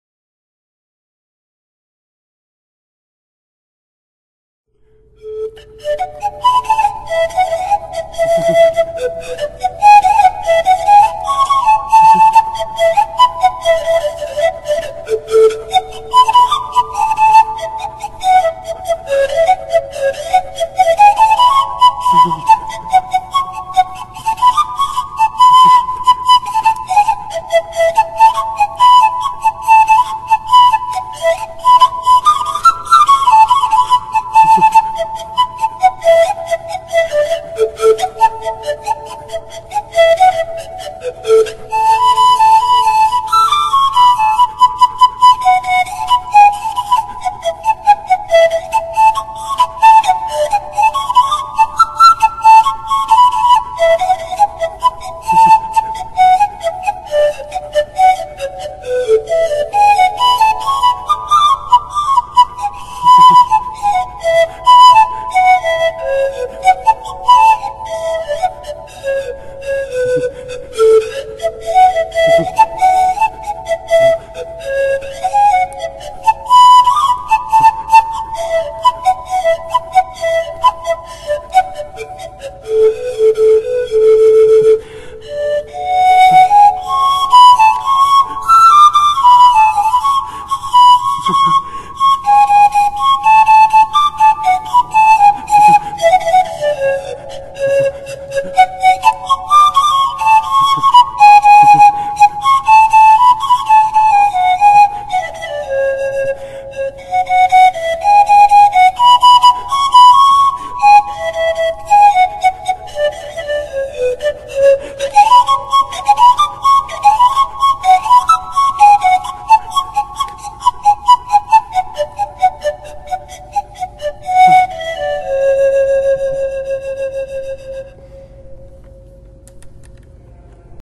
Celtic Improv (Panpipe; High audio)
Just what the title says, an improvisation in Celtic style. More the dance music type than a ballad.